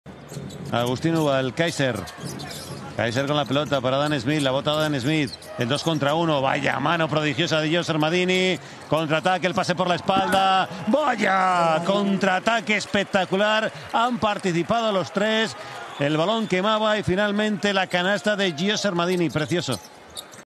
Menudo CONTRAATAQUE fulgurante de Lenovo Tenerife | Liga Endesa 2022-23
Lenadro Bolmaro para Marcelinho Huertas y este para que la reviente Gio Shermadini en el tráiler. Menudo contraataque de manual de Lenovo Tenerife.